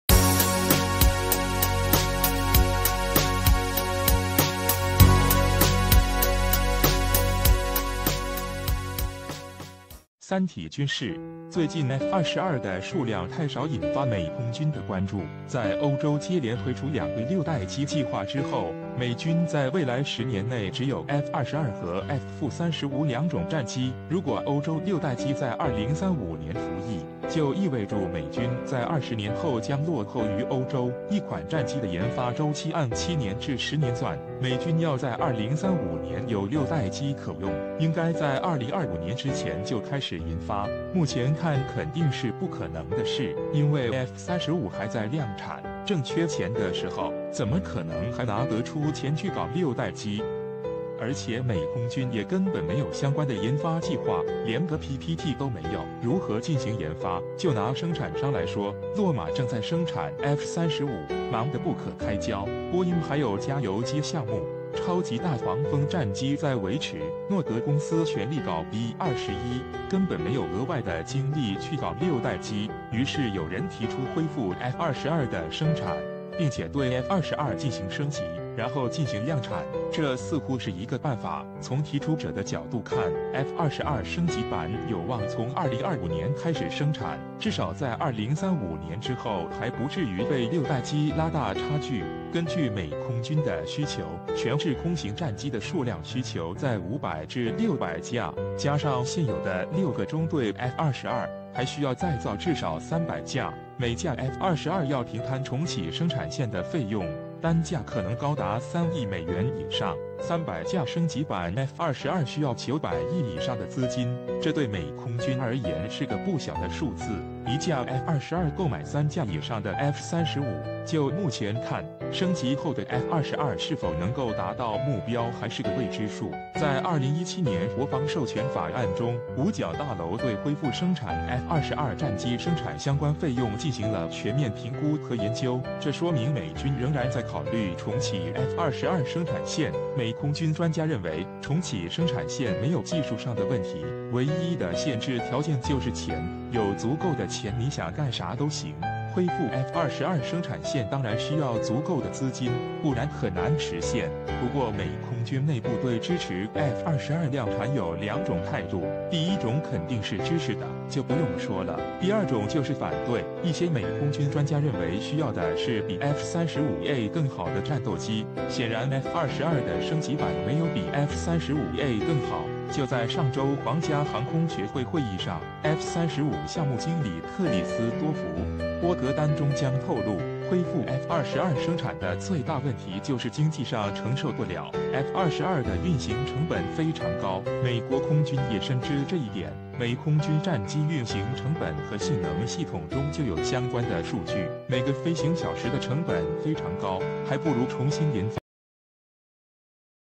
再造300架F-22戰機是否可行？美空軍專家：只有一個限制條件
三體軍事

最近F-22的數量太少引發美空軍的關注，在歐洲接連推出兩個六代機計劃之後，美軍在未來十年內只有F-22和F-35兩種戰機。如果歐洲六代機在2035年服役，就意味著美軍在20年後將落後於歐洲。一款戰機的研發周期按7年至10年算，美軍要在2035年有六代機可用，應該在2025年之前就開始研發，目前看肯定是不可能的事。因為F-35還在量產，正缺錢的時候，怎麼可能還拿得出錢去搞六代機？！

而且美空軍也根本沒有相關的研發計劃，連個PPT都沒有，如何進行研發。就拿生產商來說，洛馬正在生產F-35，忙得不可開交，波音還有加油機項目、超級大黃蜂戰機在維持，諾格公司全力搞B-21，根本沒有額外的精力去搞六代機。

於是有人提出恢復F-22的生產，並且對F-22進行升級，然後進行量產。這似乎是一個辦法，從提出者的角度看，F-22升級版有望從2025年開始生產，至少在2035年之後還不至於被六代機拉大差距。根據美空軍的需求，全制空型戰機的數量需求在500至600架，加上現有的6個中隊F-22，還需要再造至少300架。每架F-22要平攤重啟生產線的費用，單價可能高達3億美元以上，300架升級版F-22需要900億以上的資金，這對美空軍而言是個不小的數字。一架F-22購買3架以上的F-35，就目前看，升級後的F-22是否能夠達到目標還是個未知數。

在2017年國防授權法案中，五角大樓對恢復生產F-22戰機生產相關費用進行了全面評估和研究。這說明美軍仍然在考慮重啟F-22生產線，美空軍專家認為，重啟生產線沒有技術上的問題，唯一的限制條件就是錢。有足夠的錢你想幹啥都行，恢復F-22生產線當然需要足夠的資金，不然很難實現。

不過美空軍內部對支持F-22量產有兩種態度，第一種肯定是支持的，就不用說了。第二種就是反對，一些美空軍專家認為需要的是比F-35A更好的戰鬥機，顯然F-22的升級版沒有比F-35A更好。就在上周皇家航空學會會議上，F-35項目經理克里斯多福-波格丹中將透露，恢復F-22生產的最大問題就是經濟上承受不了，F-22的運行成本非常高，美國空軍也深知這一點。美空軍戰機運行成本和性能系統中就有相關的數據，每個飛行小時的成本非常高，還不如重新研發一款新的戰機。